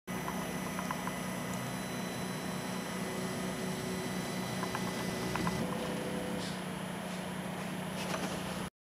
Cheap-O Homing
Demo of Cheap-O being used for homing Prusa Mendels Y-Axis